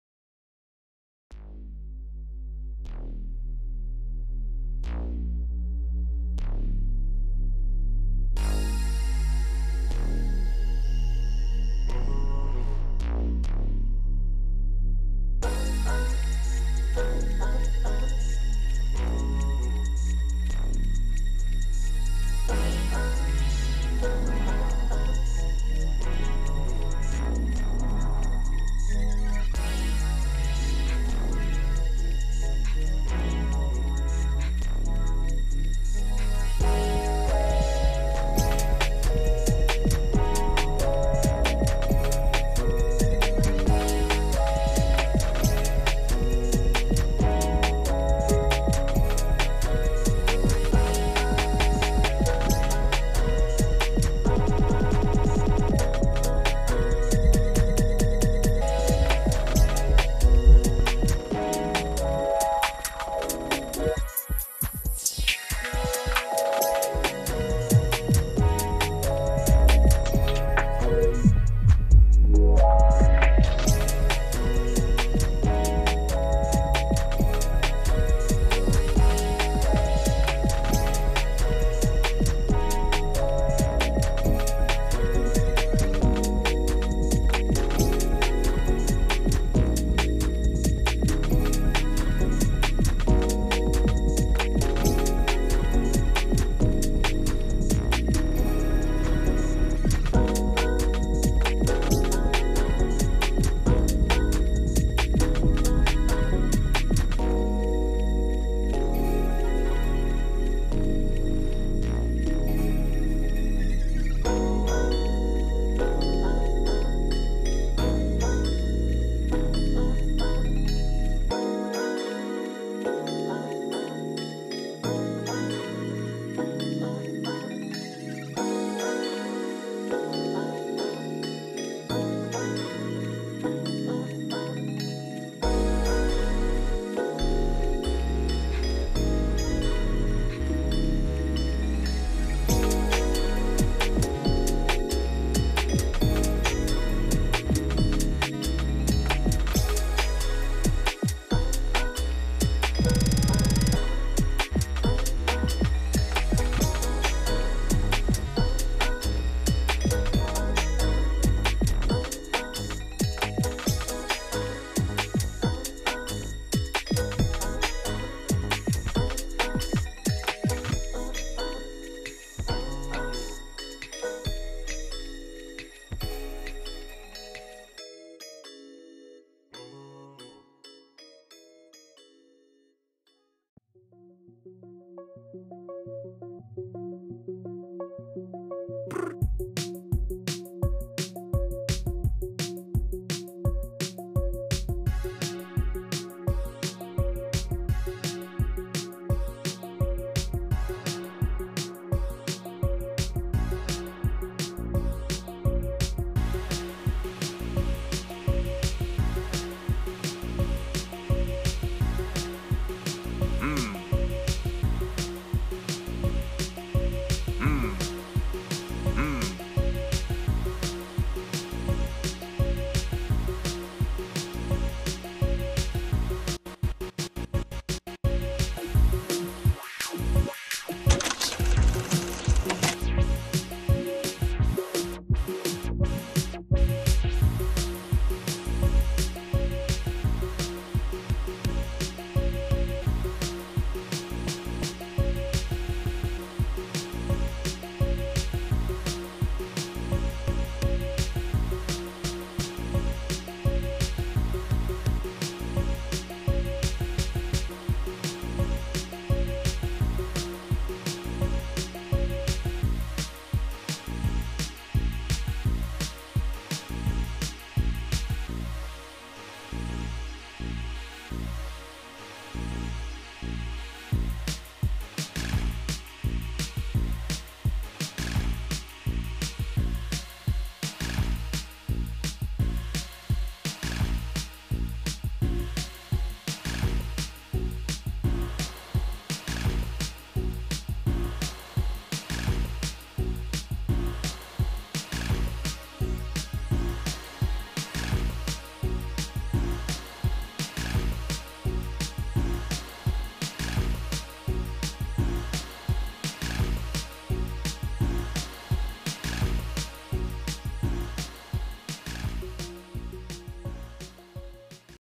Making outfit with worm bodice
The music is made by me.
It the torso from the 2023 tower!
And the 2021 or 2022 torso it the same bodice.